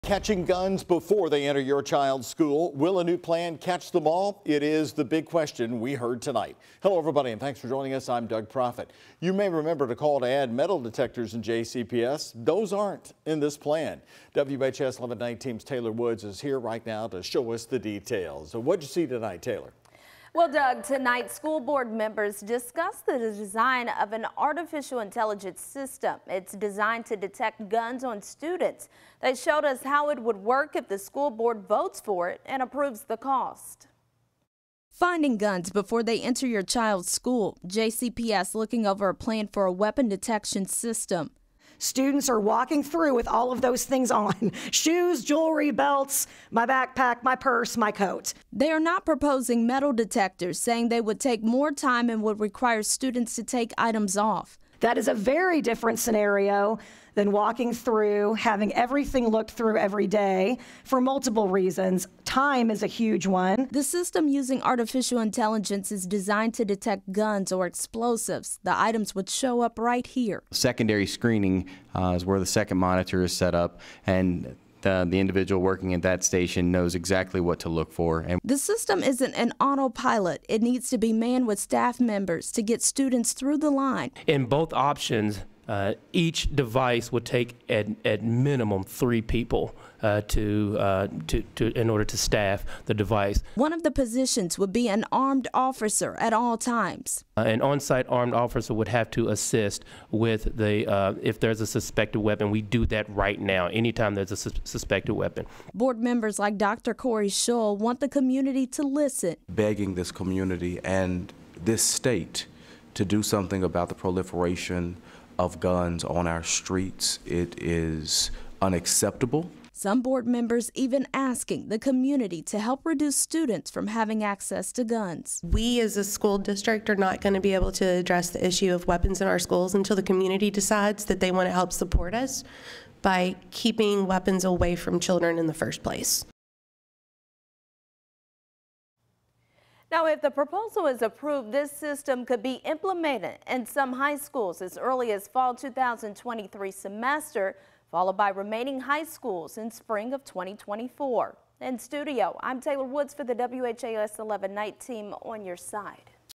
Weapon Detection Systems aims to catch guns before they enter JCPS schools
While several community members called for metal detectors in the schools, they are not present in this safety plan.